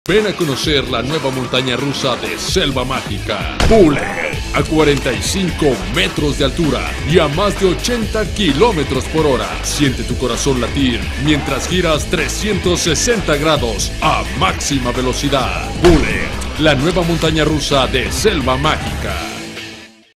BULLET - APERTURA 2013 | SELVA MÁGICA
Compartimos el último Trailer Oficial de BULLET, en su apertura 2013.
Finalmente, la apertura se concretó el Sábado 21 de Diciembre 2013 en Parque Selva Mágica.